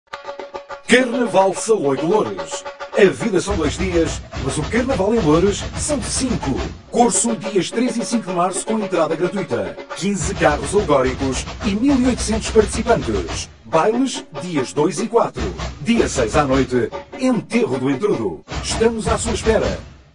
Carnaval Loures 2019
2 A 6 MARÇO
Carnaval de Loures

De 2 a 6 março, as ruas da cidade de Loures voltam a encher-se de cor e alegria para celebrar um dos maiores e mais antigos carnavais da região de Lisboa.